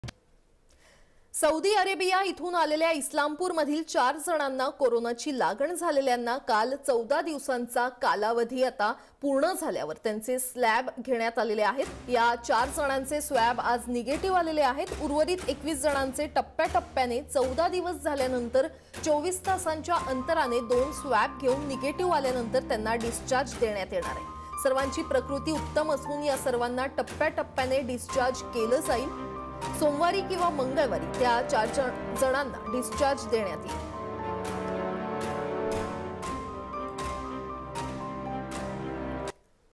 corona | इस्लामपूरमधील 4 जणांचे स्वॅब निगेटिव्ह
corona | Swab test negative of 4 persons in Islampur